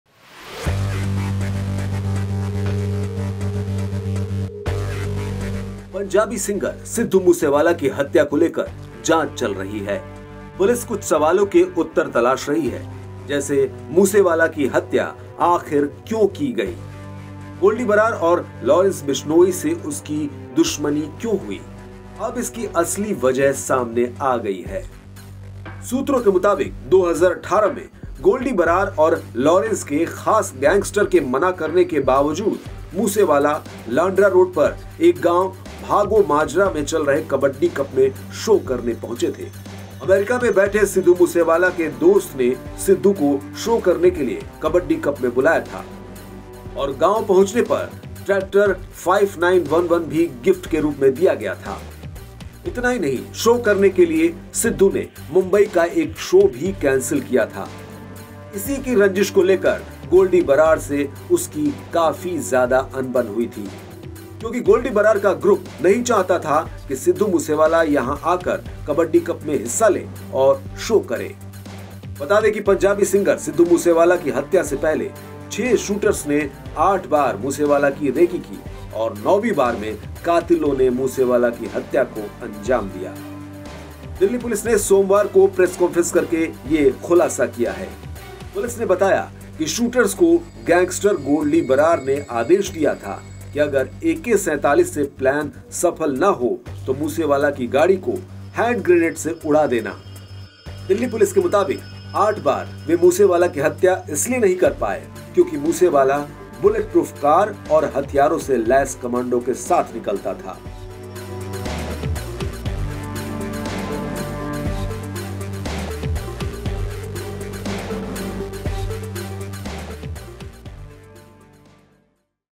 Sidhu Moosewala Murder Case: क्या थी मूसेवाला की Goldy Brar और Lawrence Bishnoi से दुश्मनी की वजह?
Sidhu Moosewala Murder Case: पंजाबी सिंगर सिद्धू मूसेवाला की हत्या को लेकर जांच चल रही है. पुलिस कुछ सवालों के उत्तर तलाश रही है. जैसे मूसेवाला की हत्या आखिर क्यों की गई? गोल्डी बरार और लॉरेंस बिश्नोई से उनकी दुश्मनी क्यों हुई. अब इसकी असली वजह सामने आ गई है. पुलिस के पुख्ता सूत्रों के मुताबिक, 2018 में गोल्डी बरार और लॉरेंस के खास गैंगस्टर के मना करने के बावजूद मूसेवाला ने लांडरा रोड पर एक गांव भागो माजरा में चल रहे कबड्डी कप में शो करने पहुंचे थे. बस इसके बाद दोनों के बीच आपसी रंजिश बढ़ गई...